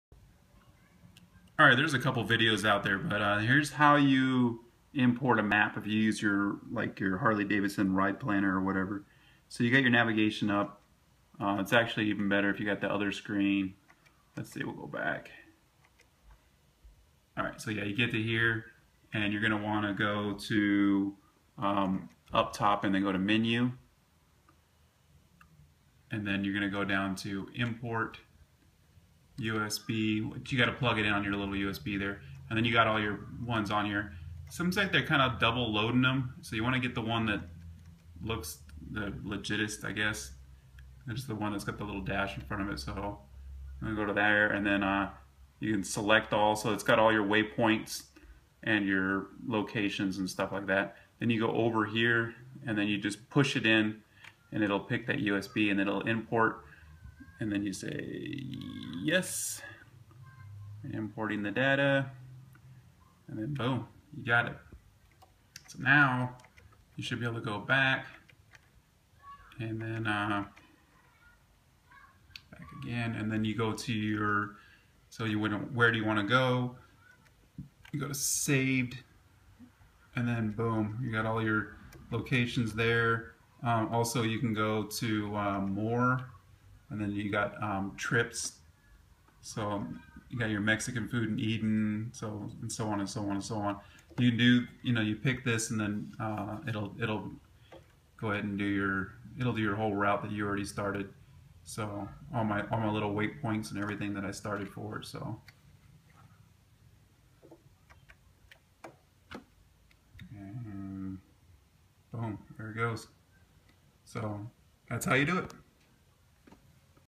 How To: Load Harley Davidson's Ride Planner on your Hog
A quickie on how to load your route you made on Harley's Ride Planner onto your 2014+ Boom Audio